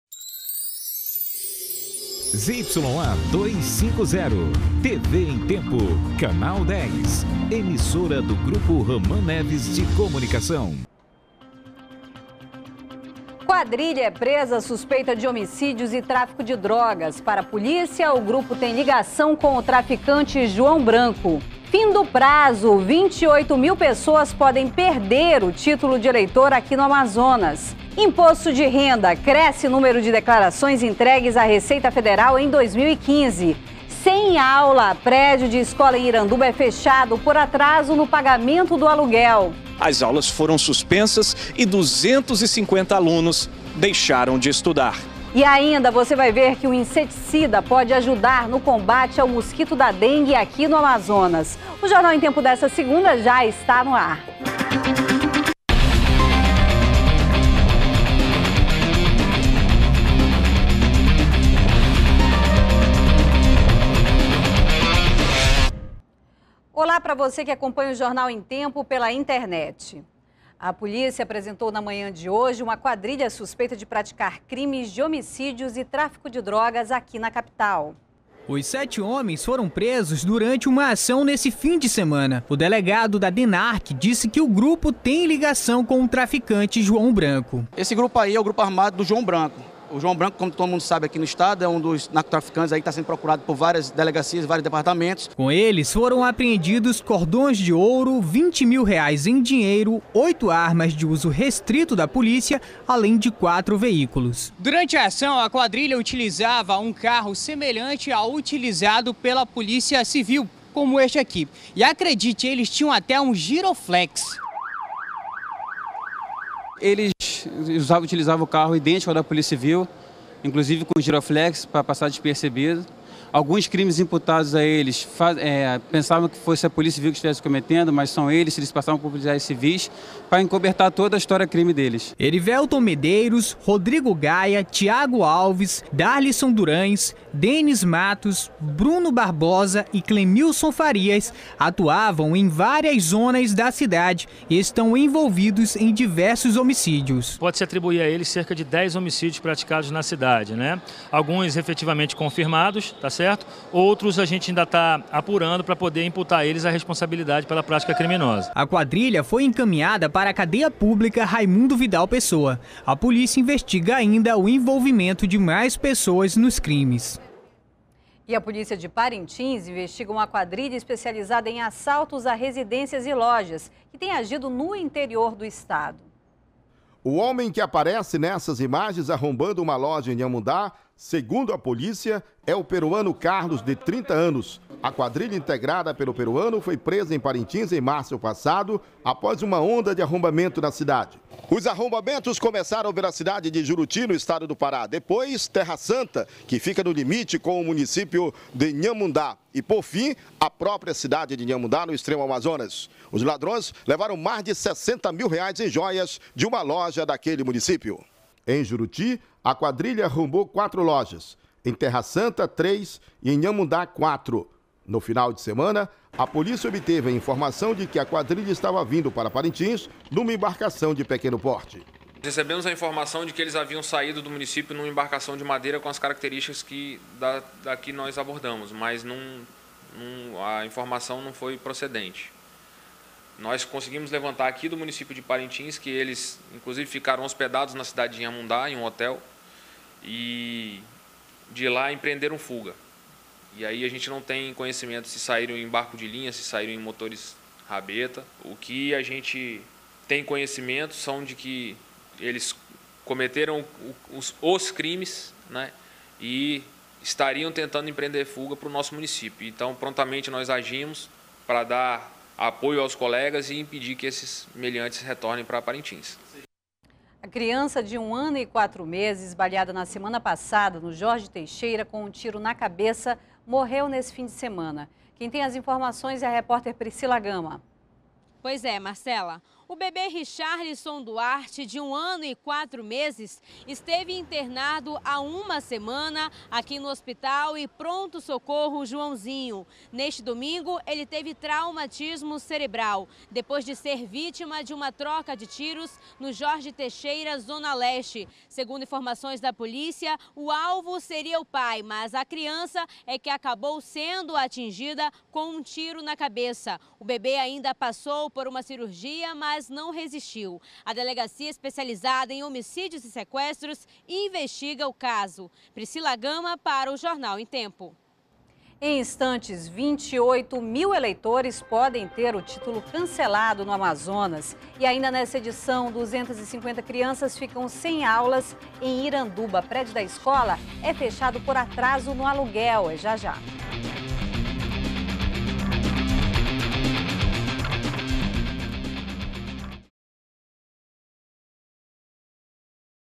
Quadrilha é presa com armas e segundo a polícia, o grupo tem ligação com o traficante João Branco
01º BLOCO JORNAL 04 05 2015 - Quadrilha é presa com armas e segundo a polícia, o grupo tem ligação com o traficante João Branco